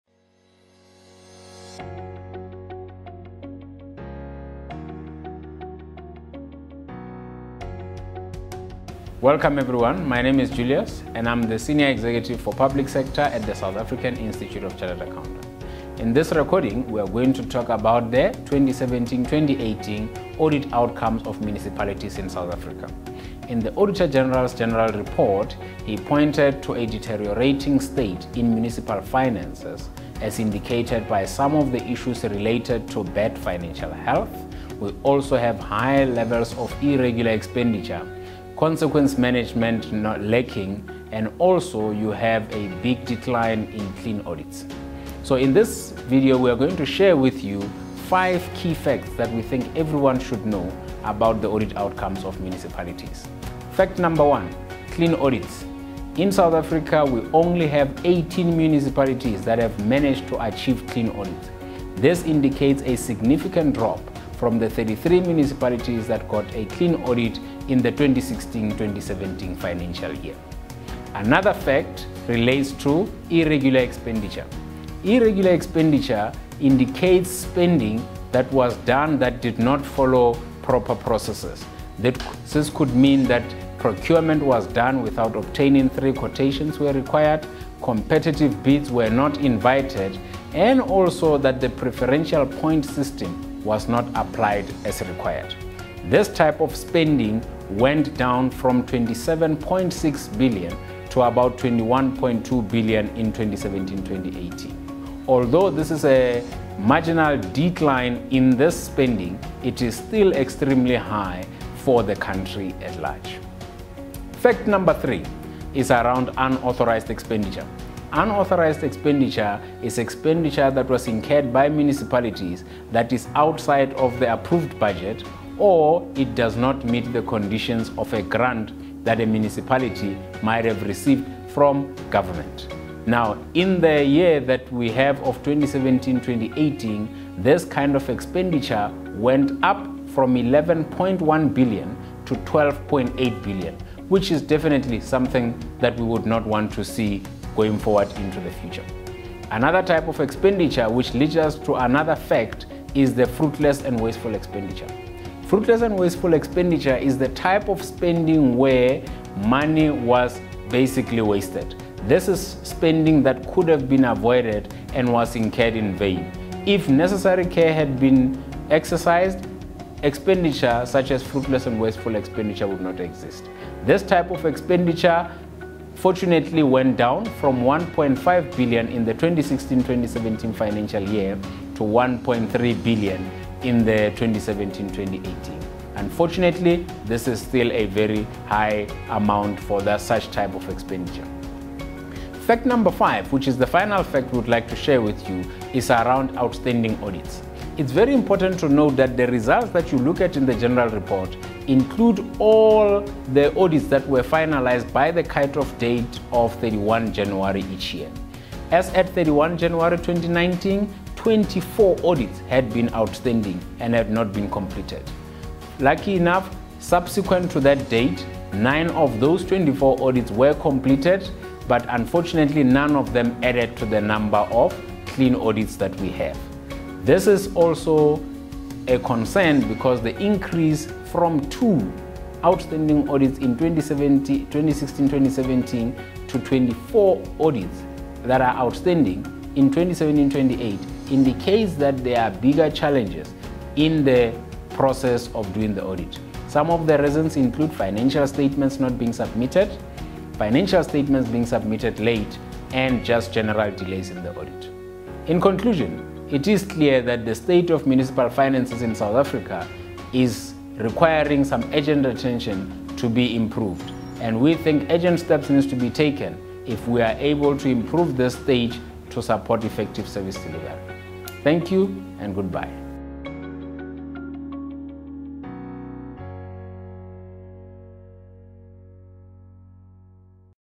Julius Mojapelo CA(SA), 2017 - 18 Municipal Audit Outcomes Quick Facts
Julius Mojapelo CA(SA), SAICA Senior Executive: Public Sector talks about the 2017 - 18 Municipal Audit Outcomes Quick Facts.